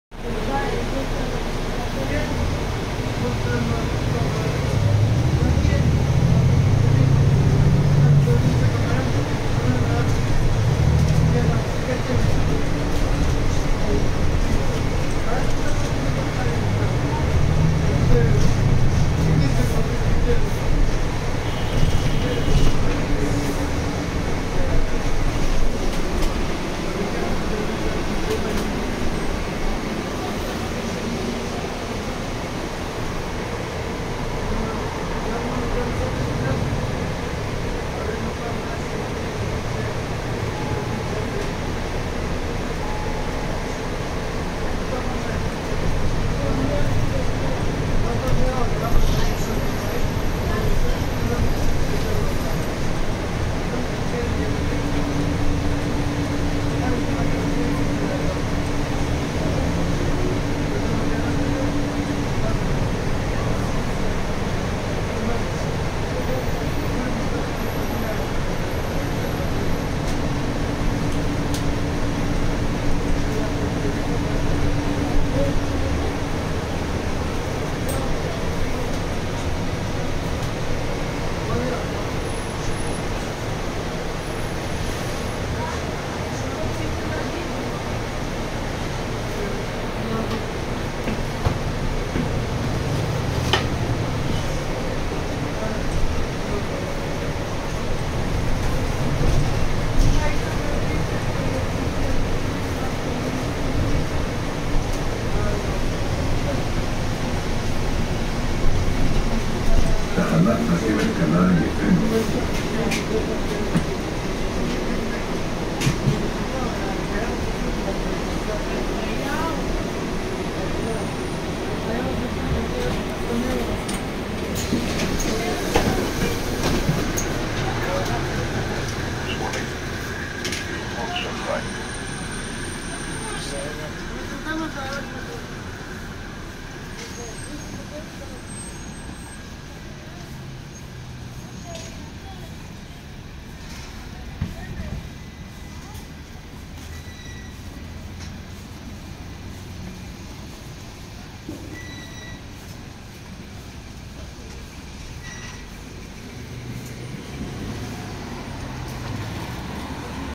Daily Life in Jerusalem Israel | Morning The City awakes - Impressions from the Bus
Thomas Huth